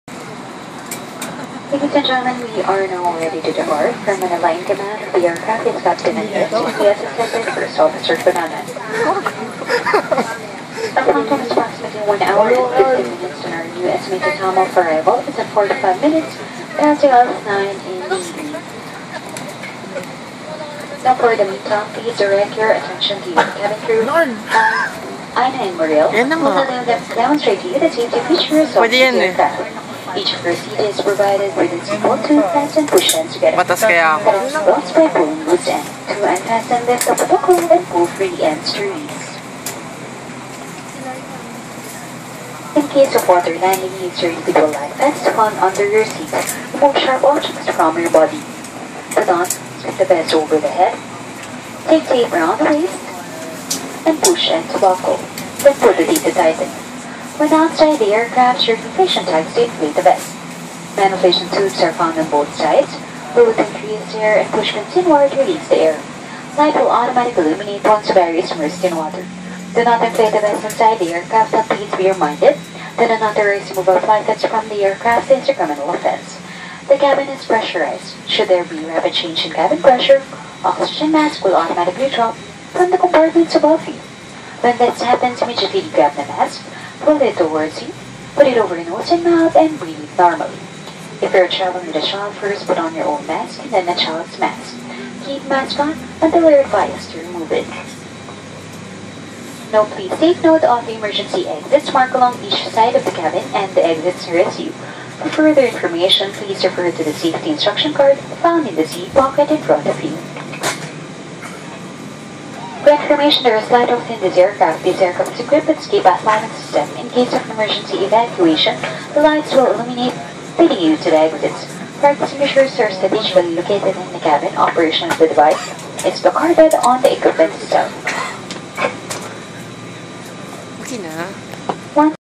✈️ Palawan Flight Safety Demo with Cebu Pacific Flight Attendant 👩‍✈️
Feel free to drop any questions or suggestions in the comments below.